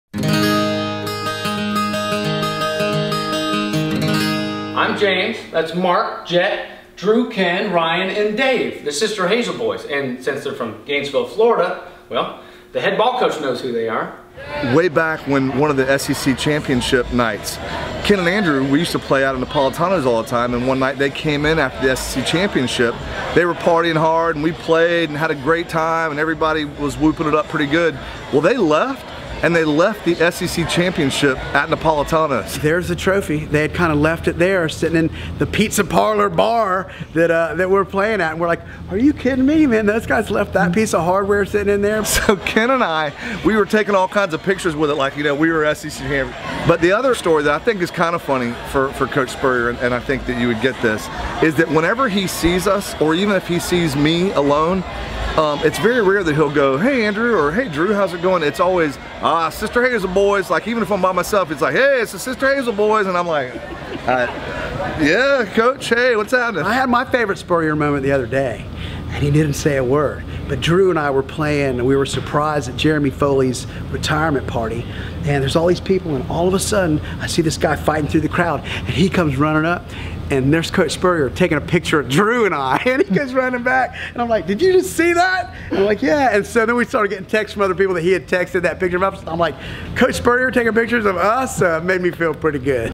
Head Ball Coach Stories: The Sister Hazel Boys
Gainesville natives, Andrew Copeland and Ken Block (From The Sister Hazel Band) share a couple of their favorite Head Ball Coach Stories.

Hank Williams Junior Jr.